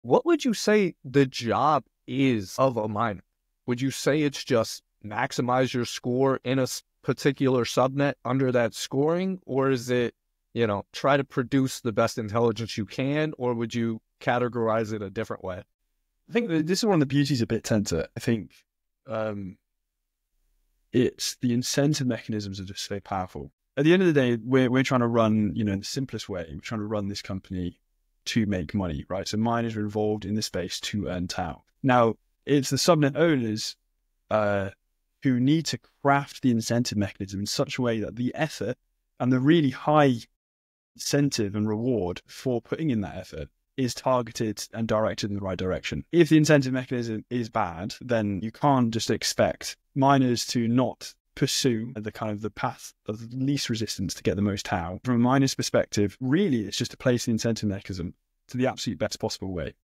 How Miners Really Work on Bittensor: Incentives, Rewards, and the Pursuit of TAO
Ever wondered what makes miners tick on Bittensor? We explore the role of incentive mechanisms in shaping miner behavior and discuss how subnet owners can design effective rewards to drive high-quality contributions. Learn how miners are motivated to pursue the path of least resistance to maximize their rewards and how subnet owners can craft incentives to align with their goals. Get a deeper understanding of the miner-subnet owner dynamic and how it drives the Bittensor ecosystem.